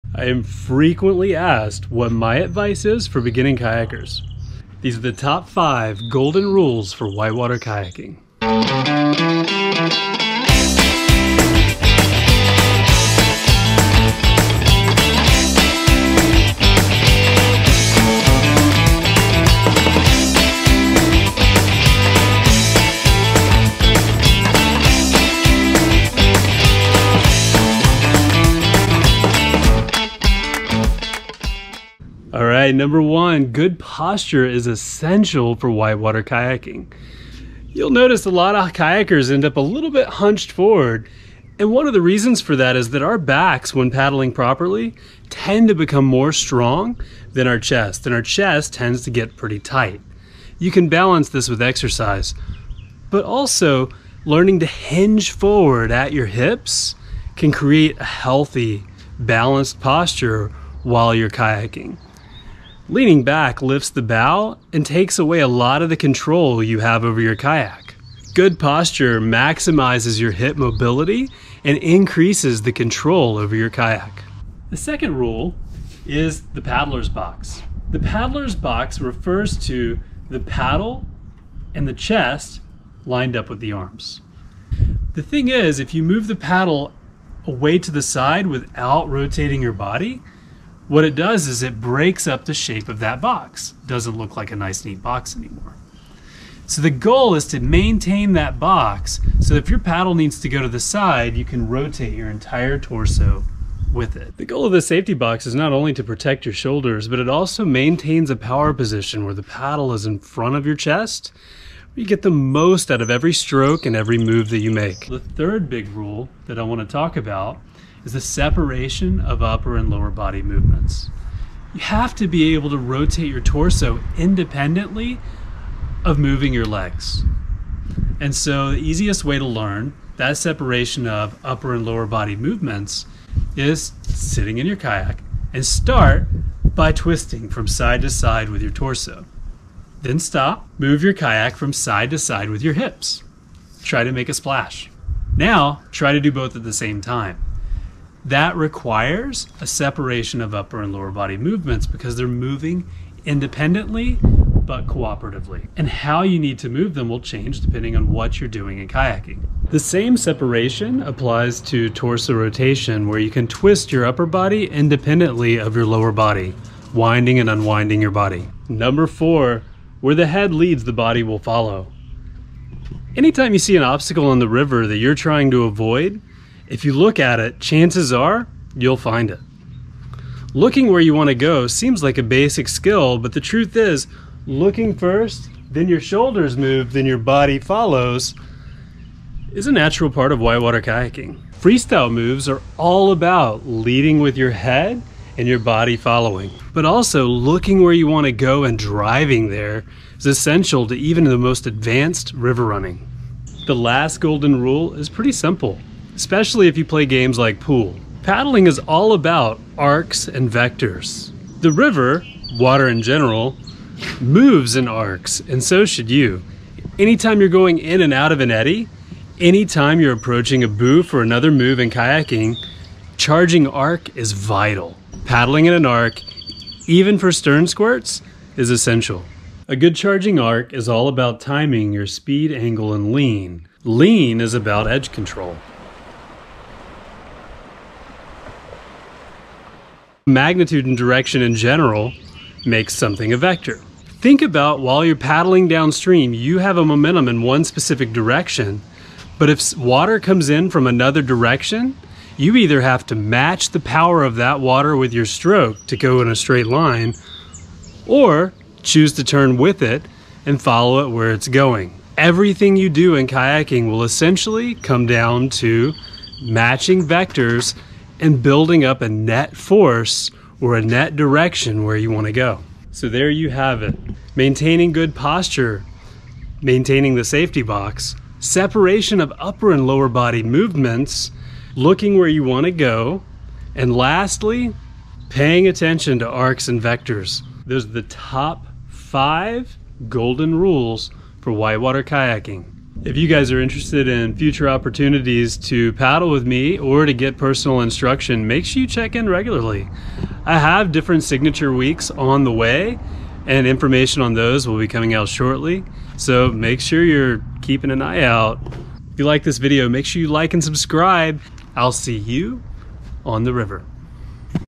5 Golden Rules For Kayaking
Quick and dirty edit, but whether you're just getting into whitewater kayaking or you're a long time river veteran and looking to raise your paddling game, these 5 golden rules are the fundamentals to get you there.  I don't necessarily think these are all inclusive, but when someone asks me what are the most important technique concepts for getting started in kayaking, these are the 5 Golden Rules for whitewater kayaking technique that come to mind:

1. Good Posture
2. The Paddler's Box
3. Separation of Upper and Lower Body Movements
4. Where The Head Leads, Your Body Will Follow
5. Charging Arc and Vectors

Several of these will require their own videos for more detail and explanation, but hopefully, these will serve a starting point to jumpstart your fundamentals.